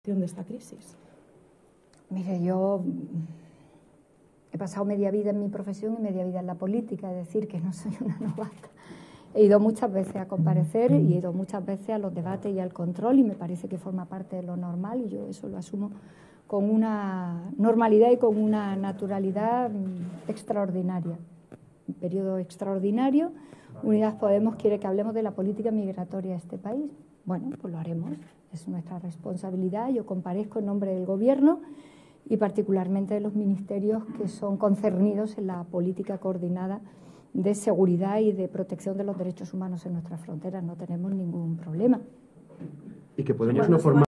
Calvo no tiene "ningún problema" en comparecer por el 'Open Arms'
La vicepresidenta del Gobierno en funciones, Carmen Calvo, ha asegurado que no tiene "ningún problema" en comparecer este jueves en el Congreso para informar de la gestión de la crisis del Open Arms, que estuvo 19 días a la deriva en el Mediterráneo con más de cien migrantes a bordo, a la espera de puerto seguro.